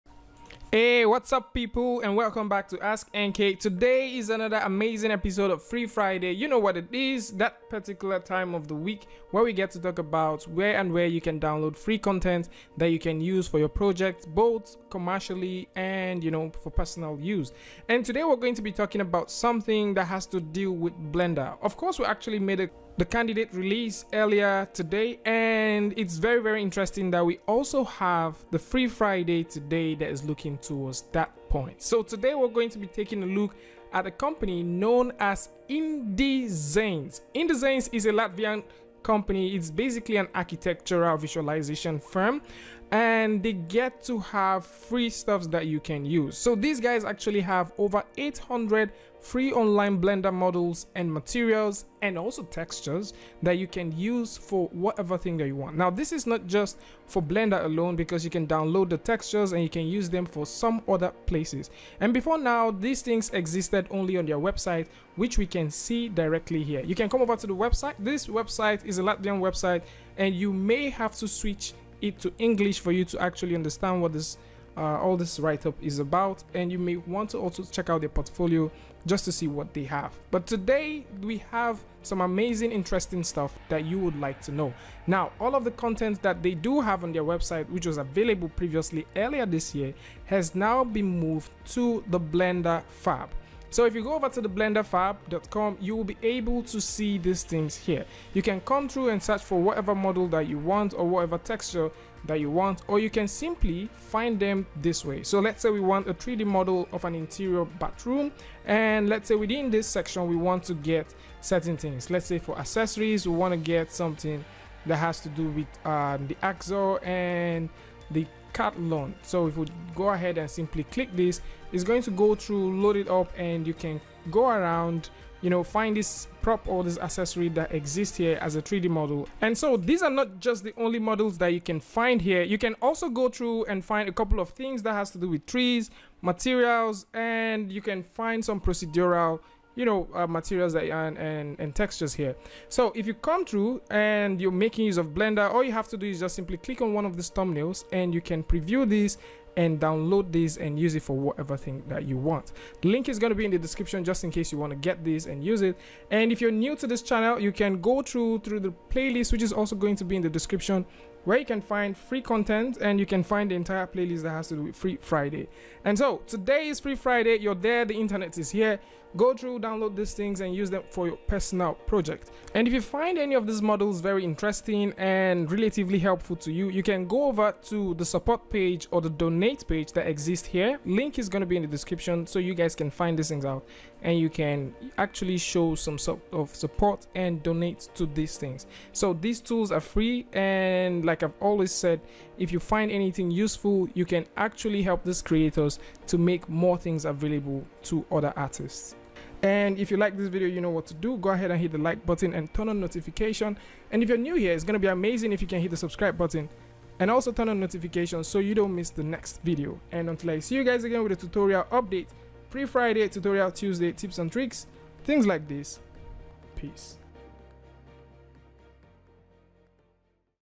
FREE 3D ASSETS & RESOURCES AT BLENDFAB - FREE FRIDAY
It's another freefriday and today we are taking a look at where we can get free blender files which can be used for commercial and non commercial projects, these range from free models, textures and procedural materials

Hey there.